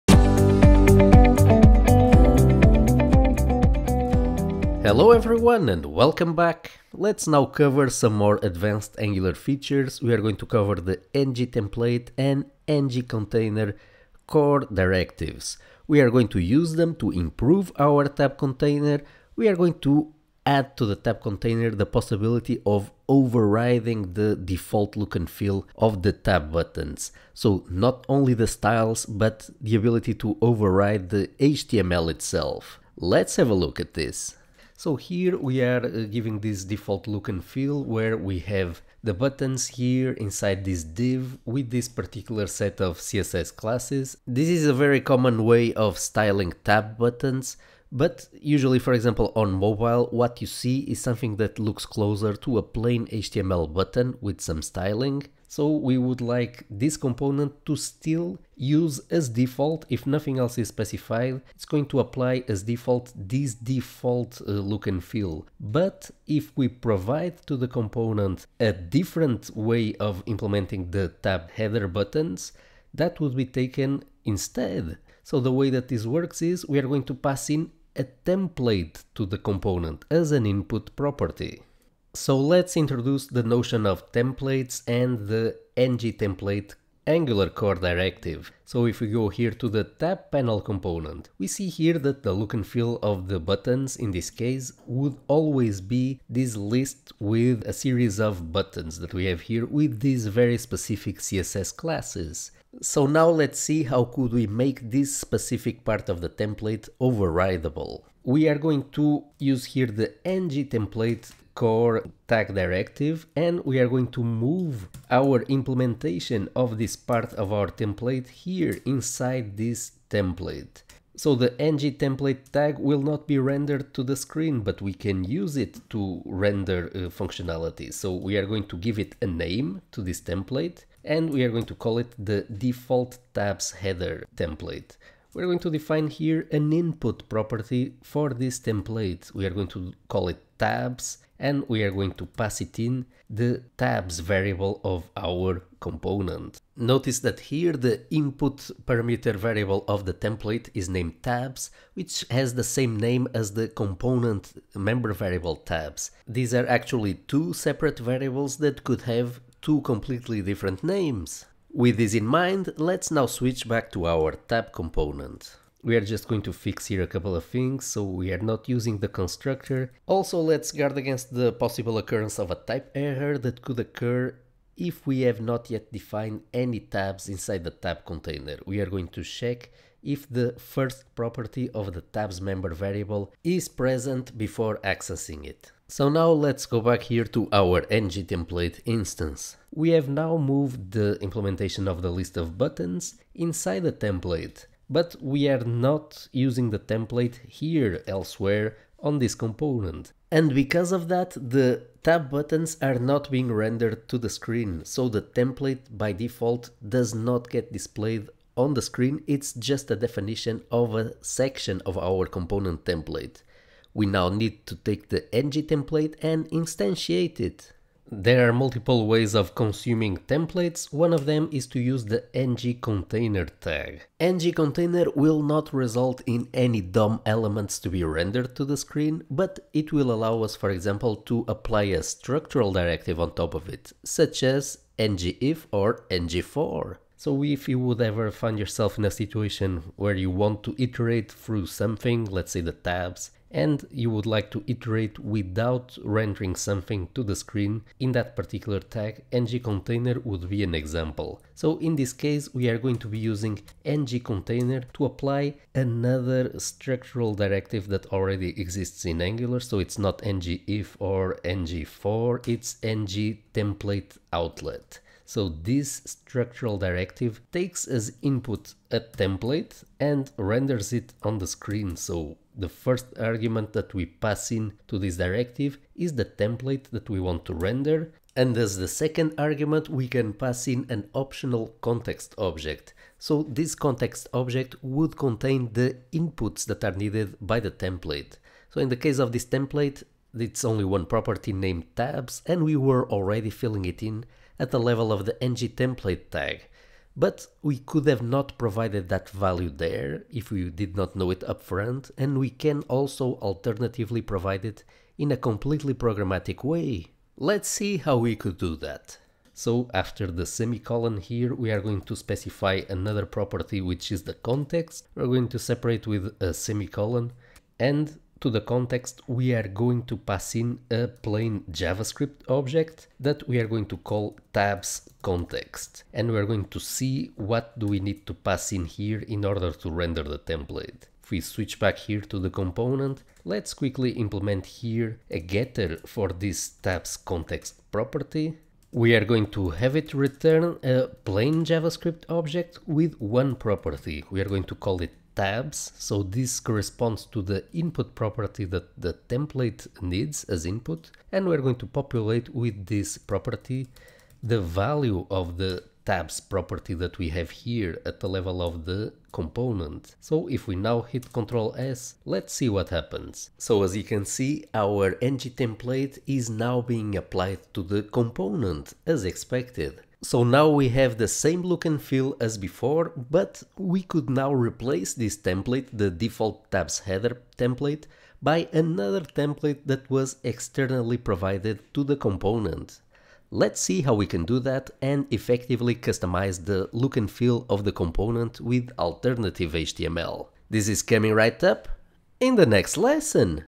💥 Learn Several Angular Advanced Features - ng-template, ng-container and ngTemplateOutlet
This video is part of the Angular Advanced Library Laboratory: Build Your Own Library Course

In this vide owe are going to continue improving our tab container component: we will add it the possibility of receiving as an input an Angular template defined using the ng-template tag.

Then we will render the template to the screen using ng-container and ngTemplate Outlet, we will show how to give the tab container buttons a default look and feel but still allow this part of the HMTL to be overriden.